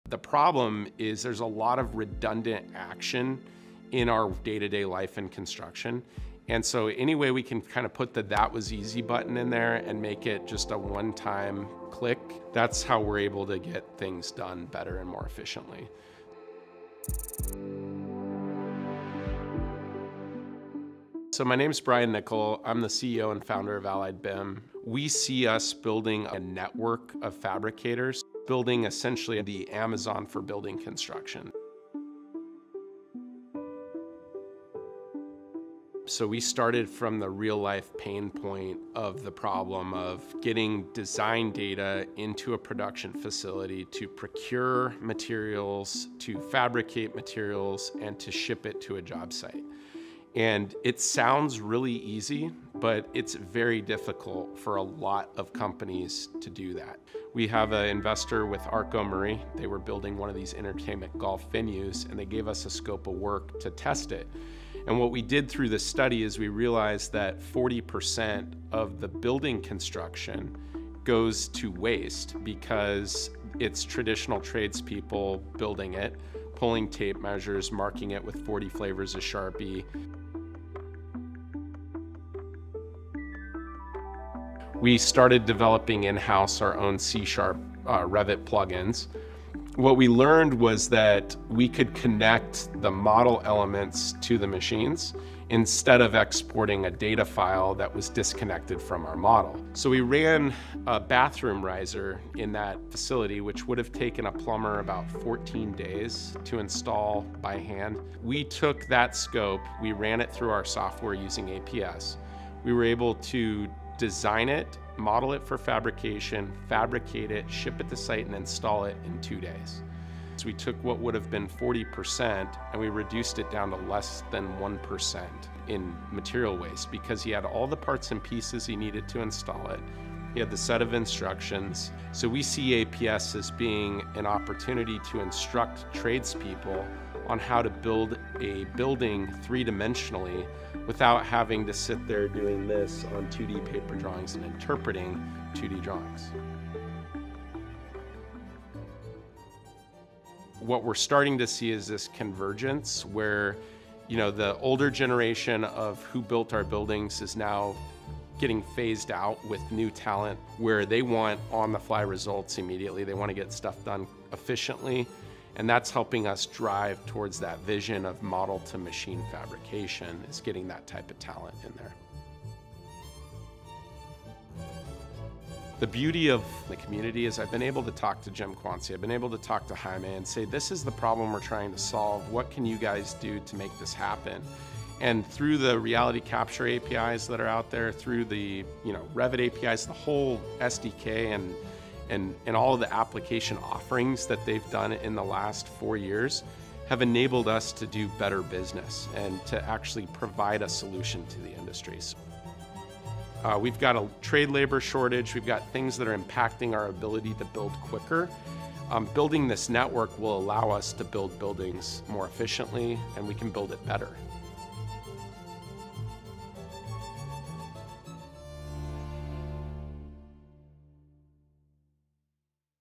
APS Success Story: Allied BIM
Allied BIM is closing the gaps between manufacturing and construction with software that connects digital drawings straight to the machine, eliminating room for error and waste.